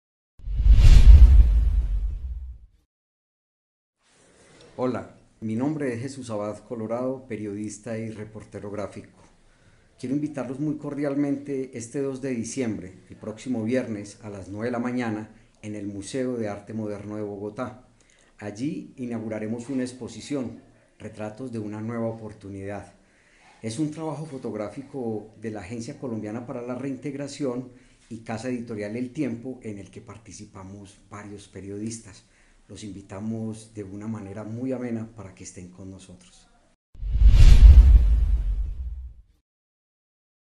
Invitación de Jesús Abad Colorado a la exposición 'Retratos de Una Nueva Oportunidad'
El periodista Jesús Abad Colorado invita a todos los colombianos a conocer el trabajo en conjunto entre la Agencia Colombiana para la Reintegración y el diario EL TIEMPO. El lugar, el Museo de Arte Moderno de Bogotá.

Los esperamos este viernes, 2 de diciembre, a las 9 de la mañana. Entrada gratis.